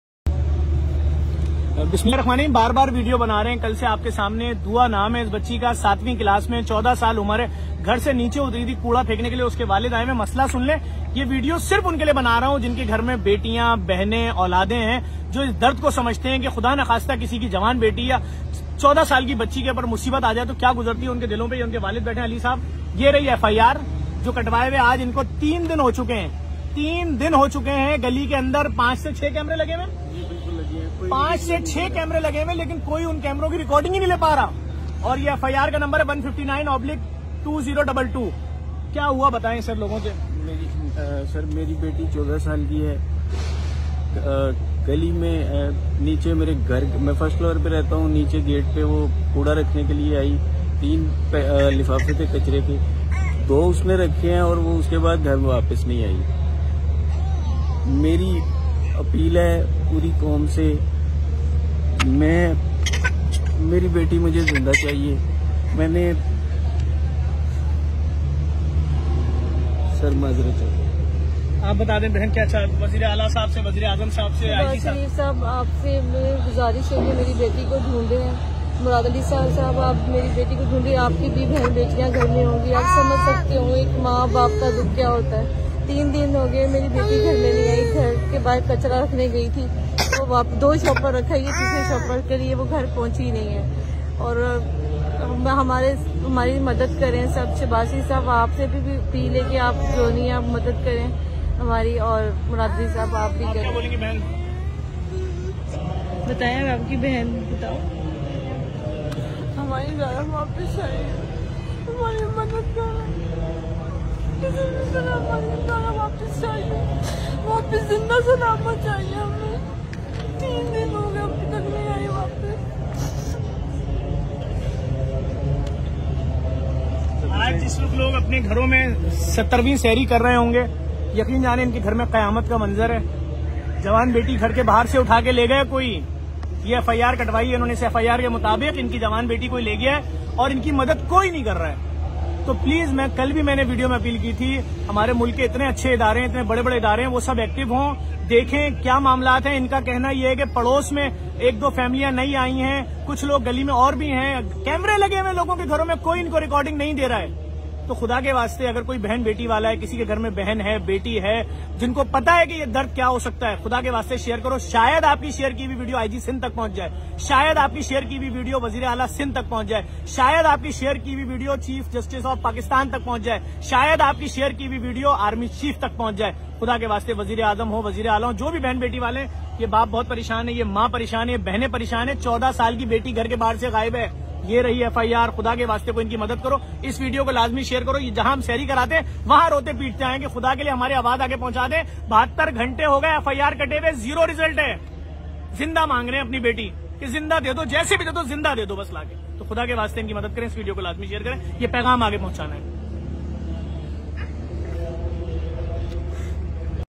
14 Years Old Girl Missing In Karachi | Share As Much As You Can | Missing Child
تلاشِ گمشدہ ! اس ویڈیو کو زیادہ سے زیادہ شئیر کریں تاکہ بچی کے والدین کی آواز چیف جسٹس تک پہنچ سکے اور بچی صحت و سلامتی کے ساتھ اپنے والدیں کو واپس مل سکے ۔